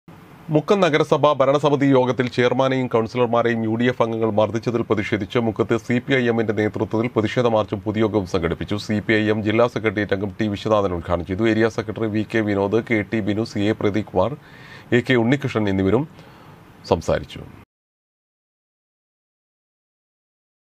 മുക്കം നഗരസഭ ഭരണ സമിതി യോഗത്തിൽ ചെയർമാനെയും കൗൺസിലർമാരെയും UDF അംഗങ്ങൾ മർദ്ദിച്ചതിൽ
മുക്കം നഗരസഭ ഭരണ സമിതി യോഗത്തിൽ ചെയർമാനെയും കൗൺസിലർമാരെയും UDF അംഗങ്ങൾ മർദ്ദിച്ചതിൽ പ്രഷേധിച്ച് മുക്കത്ത് സി.പി.ഐ എം ൻ്റെ നേതൃത്വത്തിൽ പ്രതിഷേധ മാർച്ചും പൊതുയോഗവും നടത്തി